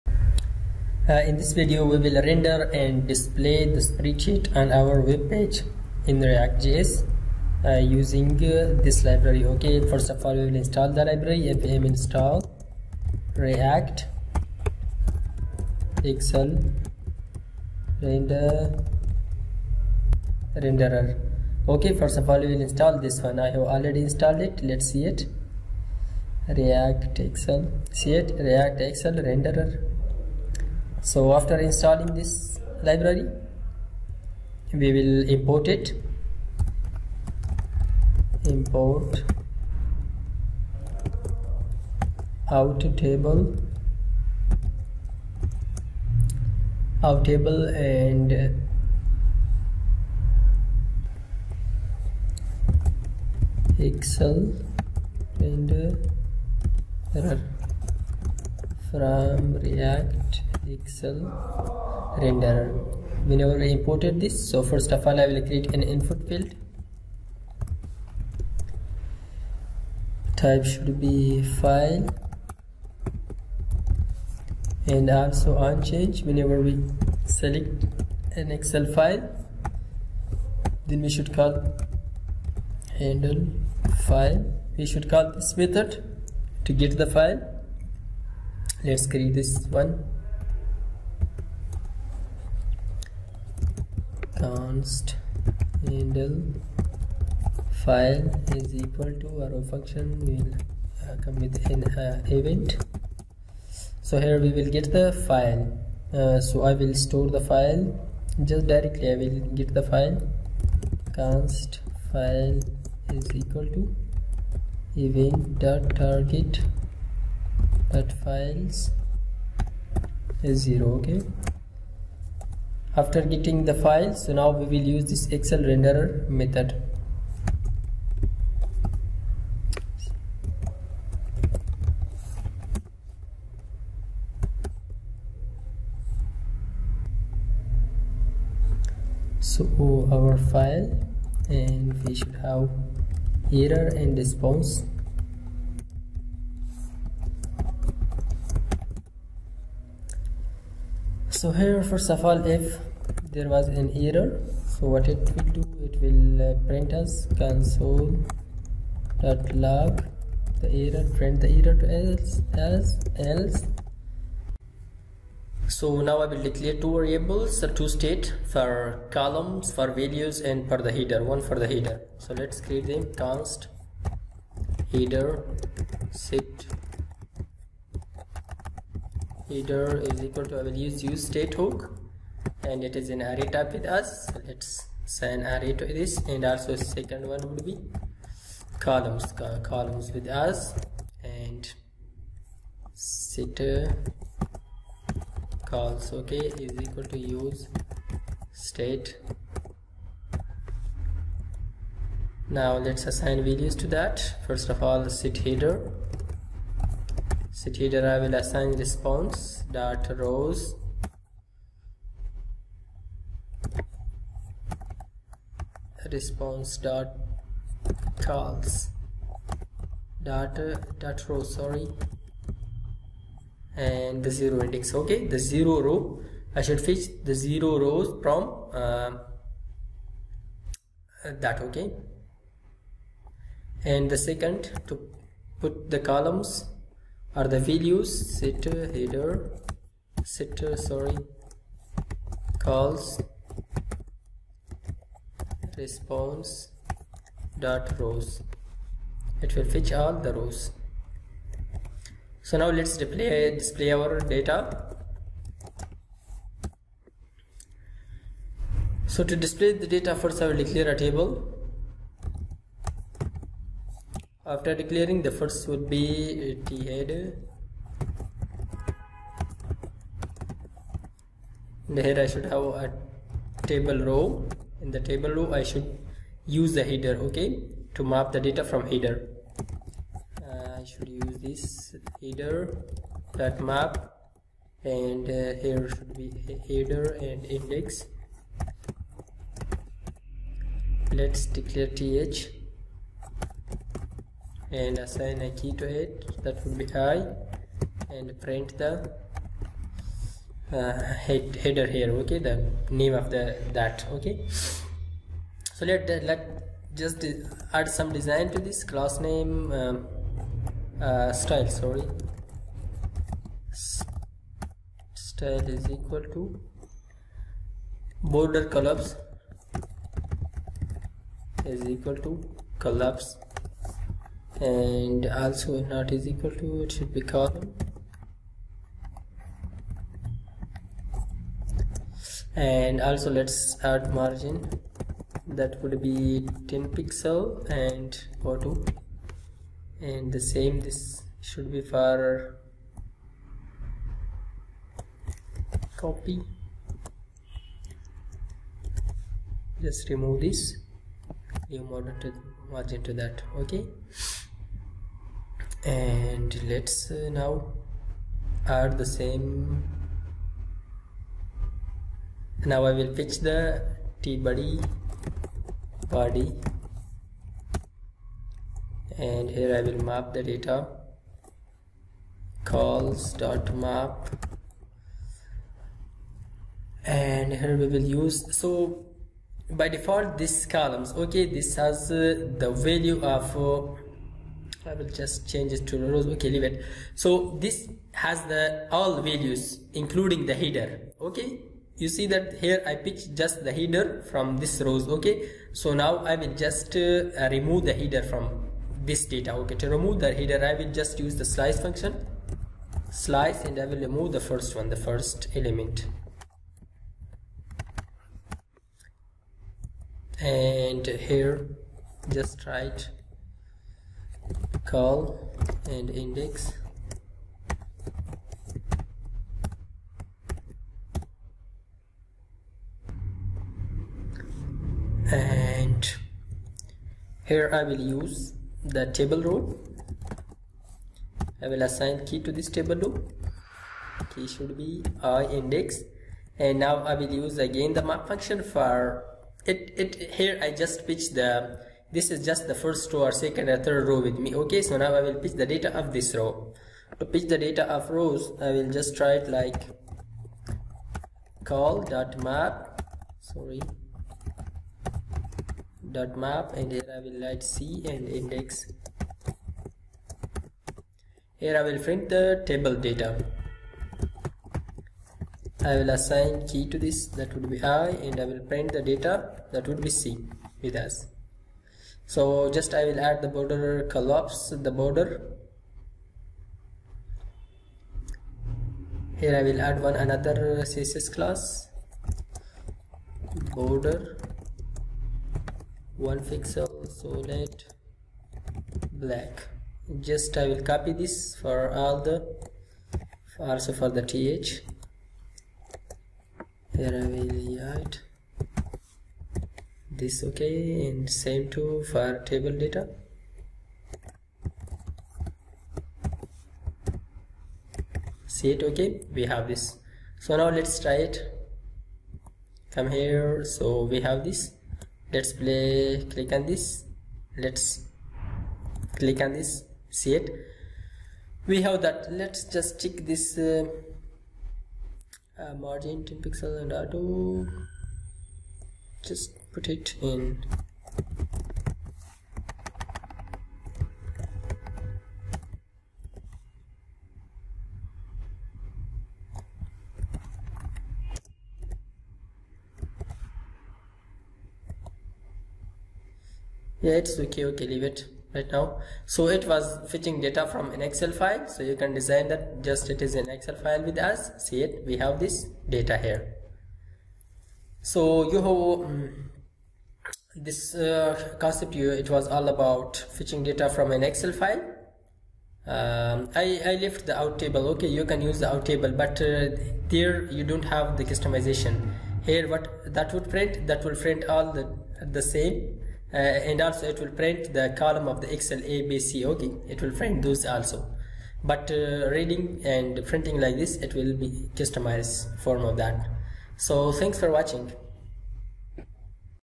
How to Render and Display Excel Spreadsheets on a Webpage with React JS | Read Excel in React
This video is about How to Read and Display Excel Spreadsheets on a Webpage with React JS | Read Excel in React. To read and parse excel files in react js we will install a package by the name of react-excel-renderer. We will use ExcelRenderer to parse the excel file.